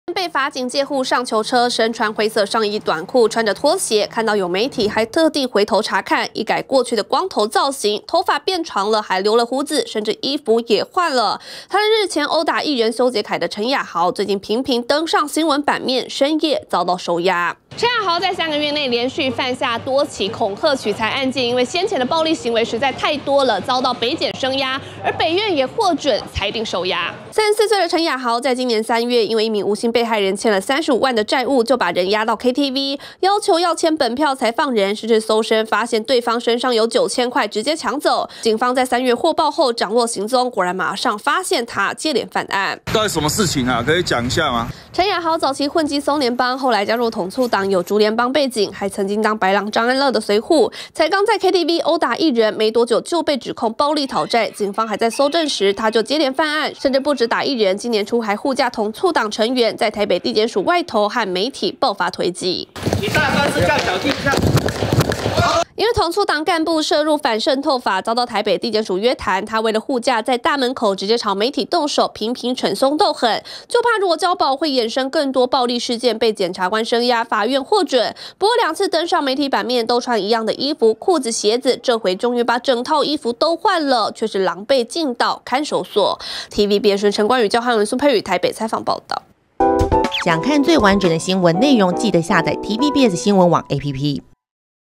「陳亞豪」列首惡遭收押 曾揍修杰楷.王柏傑｜TVBS新聞
持續追蹤，這次犯案的主嫌陳亞豪，是統促黨成員，今年三月才涉嫌毆打藝人修杰楷，事後雙方和解，也曾經在法院和記者發生推擠，半年來他鬧事至少三次登上媒體版面，由於他涉嫌多起暴力事件，這回也遭法院裁定羈押，避免再次鬧事。